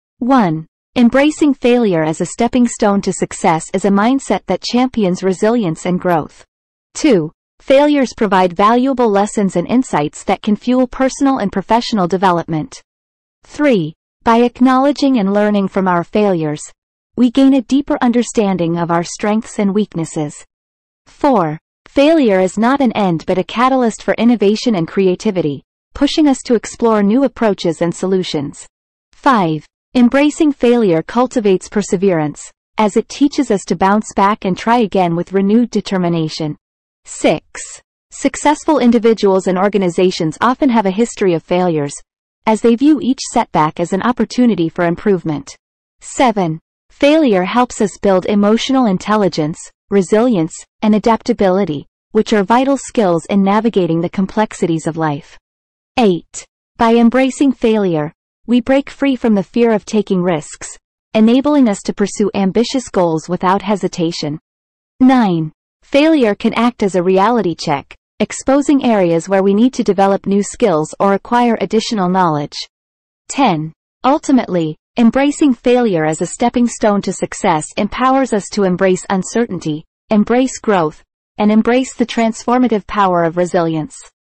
embracing failure and learning from mistakes | how embracing failure accelerated business growth
embracing failure and learning from mistakes  | how embracing failure accelerated business growth (In this inspiring video, learn the transformative power of embracing failure as a stepping stone to success. We explore the mindset shift necessary to turn setbacks into opportunities for growth and achievement. Discover valuable insights, practical strategies, and personal anecdotes that will empower you to navigate through failure with resilience and determination. By reframing failure as a valuable teacher, you can unlock hidden potential, gain valuable lessons, and ultimately propel yourself closer to success. Don't let fear of failure hold you back - embrace it, learn from it, and use it as a catalyst for personal and professional growth. Join us on this journey of resilience and discover the path to embracing failure and achieving extraordinary success.)